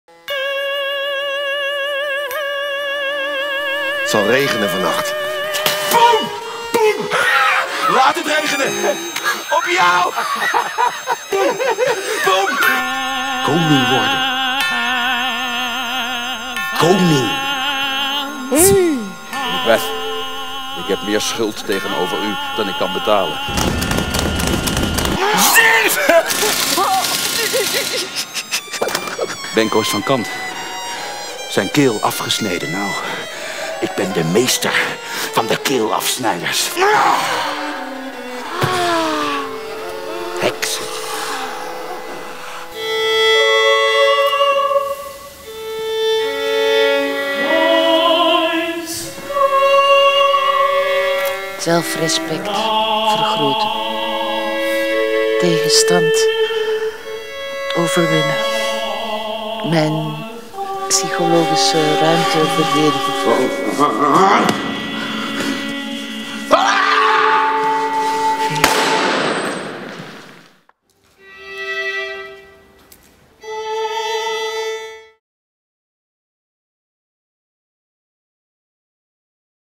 Trailer Macbeth 12|13 subtitled - Toneelgroep Amsterdam
General Macbeth (Fedja van Huêt) receives a prophecy from a trio of witches that one day he will become King of Scotland. Consumed by ambition and spurred to action by his wife, Macbeth murders King Duncan and takes the throne for himself. His reign is racked with guilt and paranoia, and he soon becomes a tyrannical ruler as he is forced to commit more and more murders to protect himself from enmity and suspicion. The bloodbath swiftly takes Macbeth and Lady Macbeth (Chris Nietvelt) into realms of arrogance, madness, and death.

Director Johan Simons (1946) considers Macbeth to be a study of violence. Not a psychological but a genetic one. Simons: 'Where does the story take place? Obviously not in a fairytale landscape, more likely a shooting range. It is an experiment. A conscious choice for evil, for true bloodlust, lying deep inside all of us, genetically predetermined.' Simons is intendant of the Munich Kammerspiele.